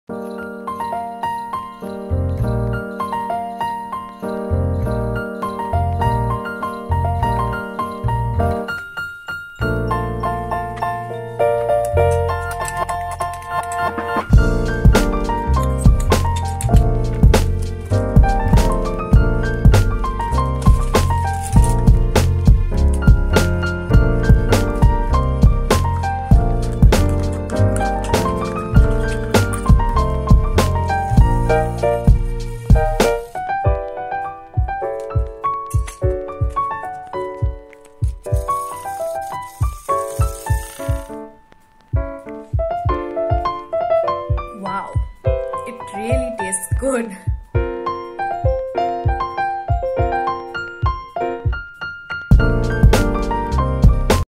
HOT CHOCOLATE under ONE MINUTE for lazy people
It's ChristmasEVE ❄️🎅and I am craving for hot choclate and hot hot kulhad wali chai. Aaahaha!! If you know the emotion...you wouldknow it ✨.
And here's Hot chocolate under one minute! Although by looks it looks more like bournvita 😂😂 but I must say it is very choclatey ...I loved loved it. especially when I don't want to cook anything ❄️